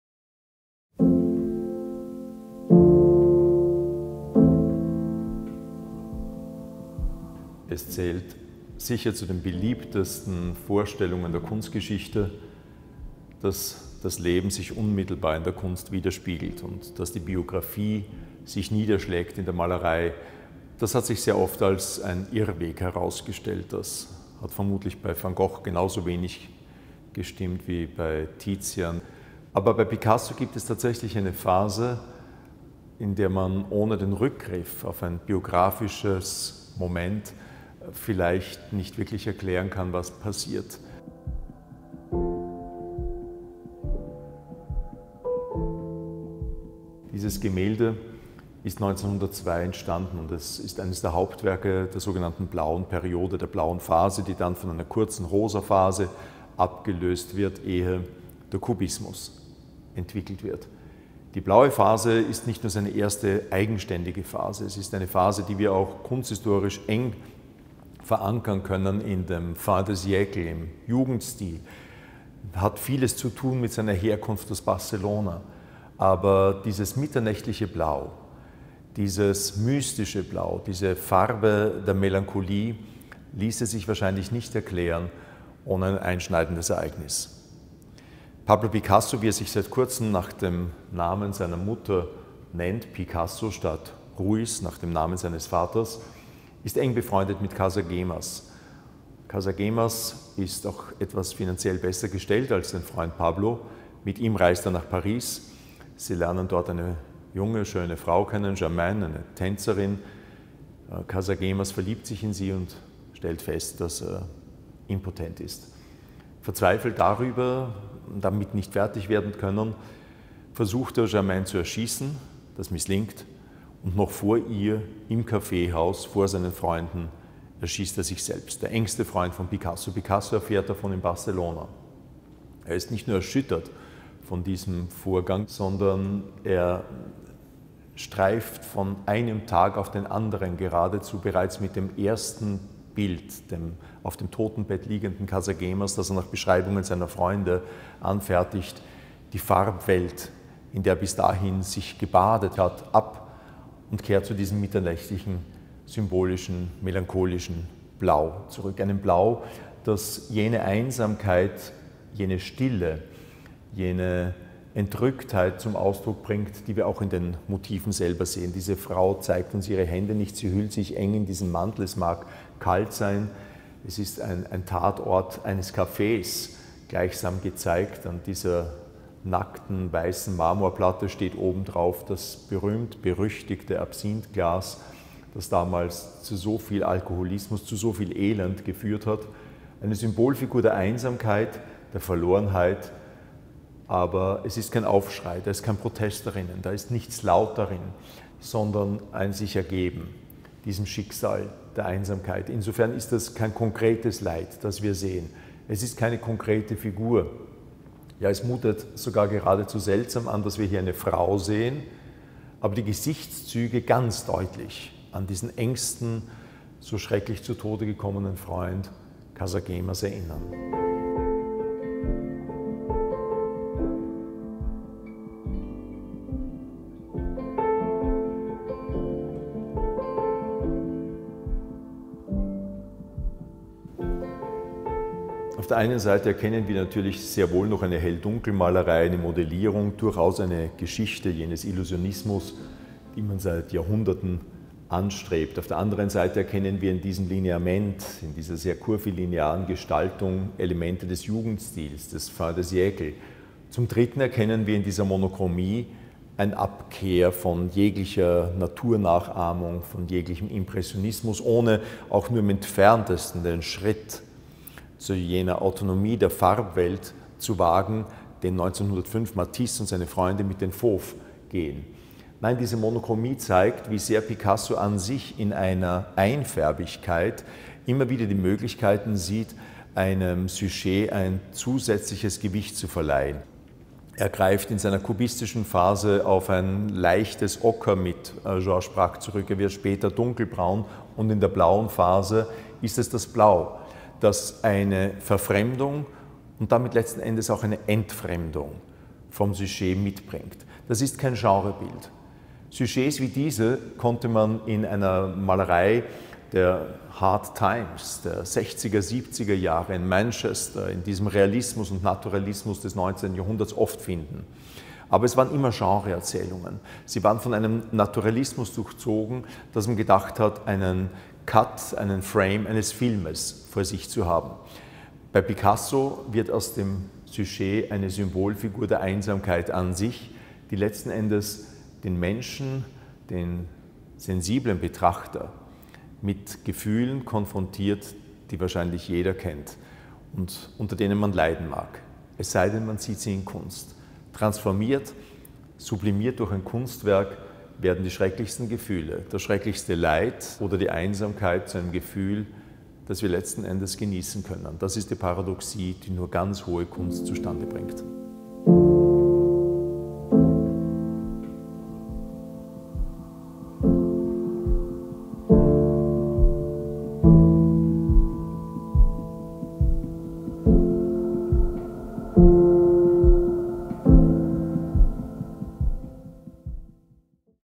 Pablo Picasso | THE SLEEPY DRINKER
What brought Picasso to paint THE SLEEPY DRINKER? And why did Picasso start his Blue Phase? The ALBERTINA MUSEUM director, Klaus Albrecht Schröder, speaks about how the first years in Paris deeply influenced Pablo Picasso's art works at the time.